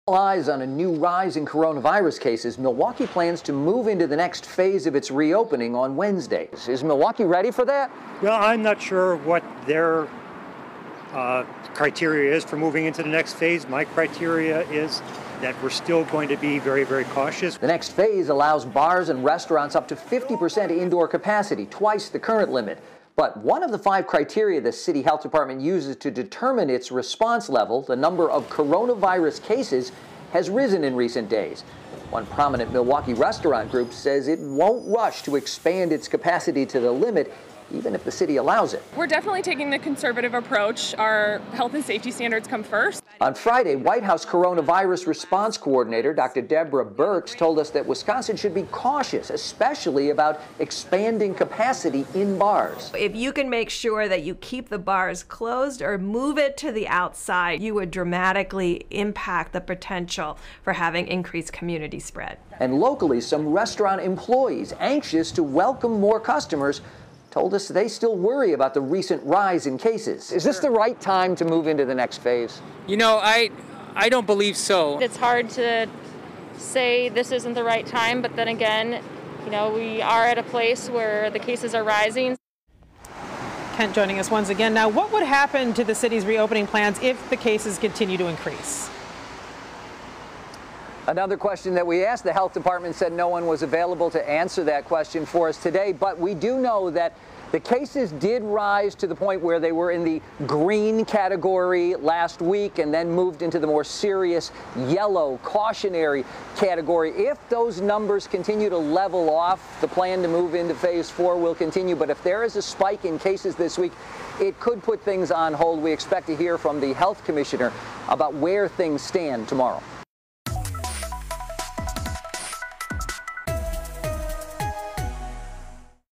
City won't yet say if spike in coronavirus cases could halt reopening plans
A recent spike in coronavirus cases across Wisconsin comes just as the city of Milwaukee plans to move into the next phase of it's reopening this week.